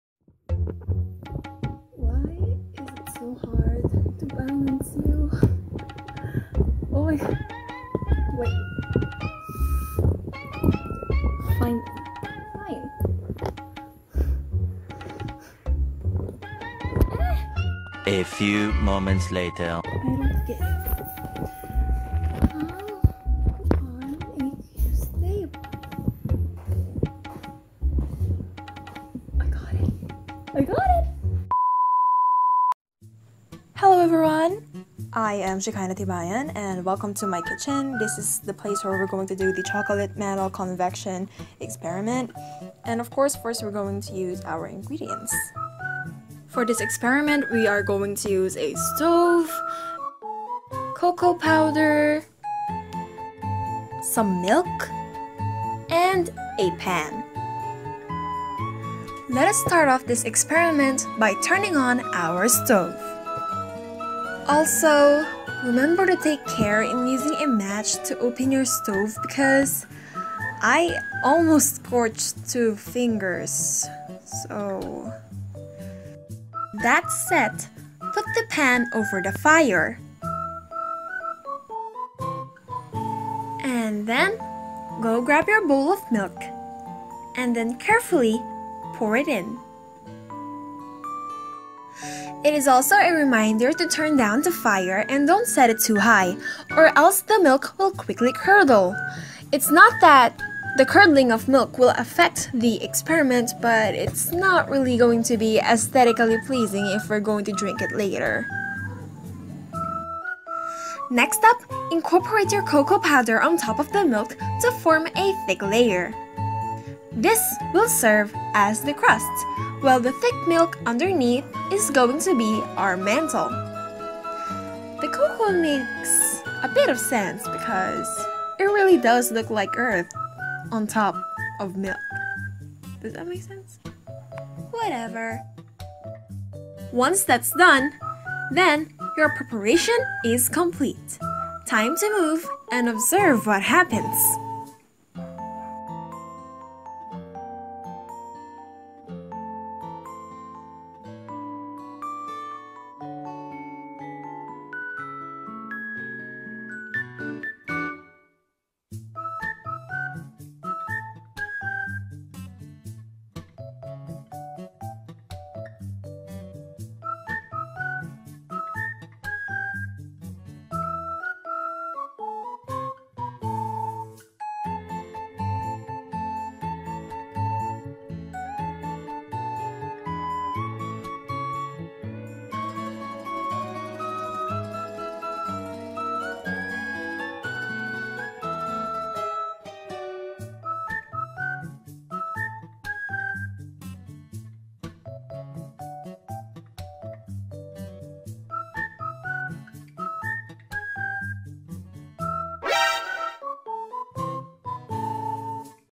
The Chocolate Mantle Convection Experiment!
Hi, I am Shekainah Tibayan of 11 - ICT Torvalds doing a performance  task in our Earth & Life Science!